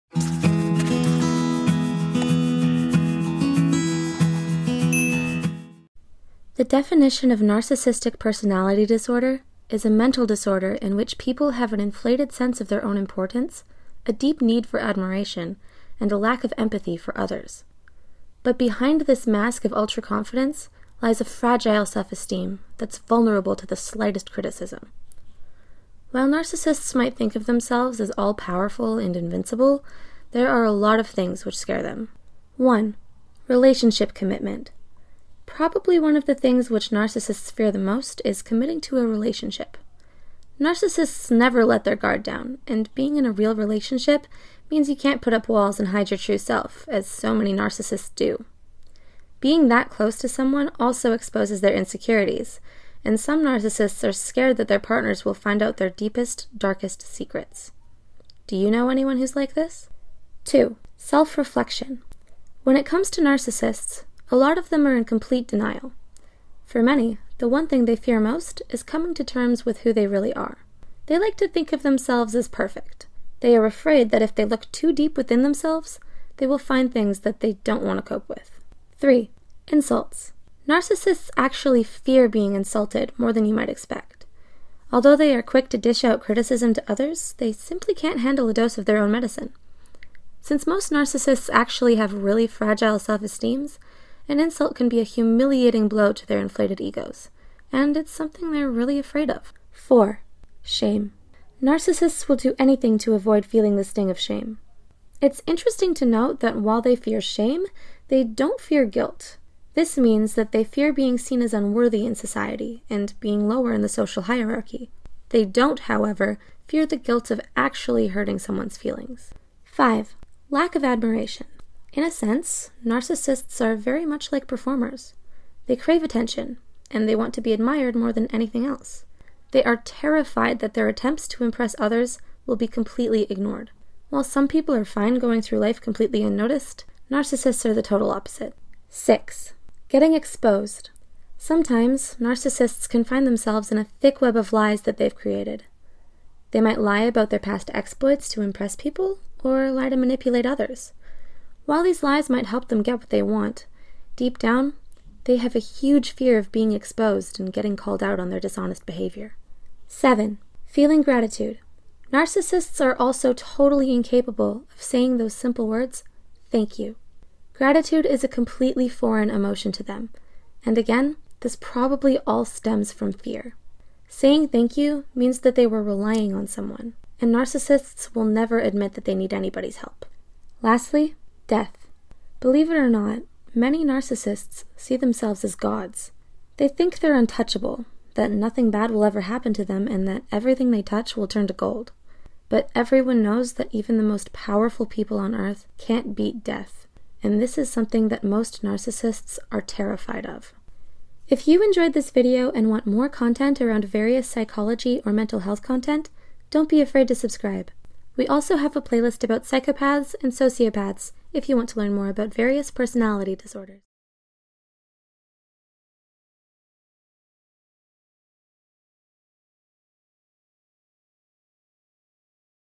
8 Things Narcissists Are Secretly Afraid Of
The definition of narcissistic personality disorder is: “A mental disorder in which people have an inflated sense of their own importance, a deep need for admiration and a lack of empathy for others. But behind this mask of ultraconfidence lies a fragile self-esteem that’s vulnerable to the slightest criticism.” While Narcissists might think of themselves as all-powerful and invincible, there are a lot of things that scare them…

Script: Elliot Figueira
Voice Over: Star Martin
Animator: Jin Kim
YouTube Manager: Wendy Hu

Intro: Surey Camacho

Community Manager: Priscilla Cha, Nicole Pridemore
Producer: Psych2Go